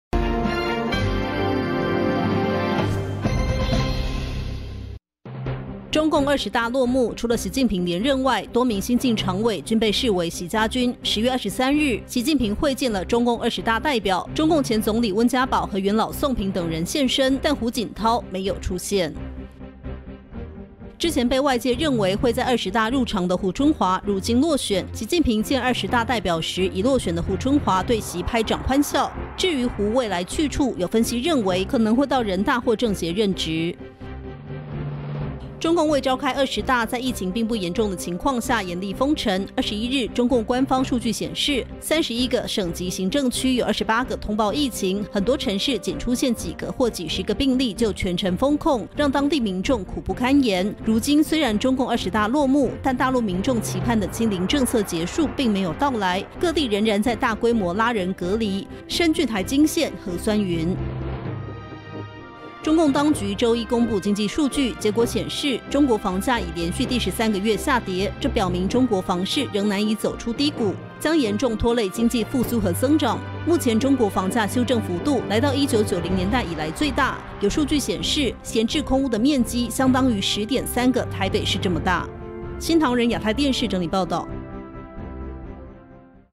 二十大後清零繼續 大陸多地大規模拉人隔離｜中國一分鐘
🔸習率一眾高官見二十大代表 胡錦濤未現身
🔸胡春華落選後對習拍掌歡笑 分析：或退居二線
🔸二十大後清零繼續 大陸多地大規模拉人隔離
🔸中國房市危機持續 房價連續13個月下跌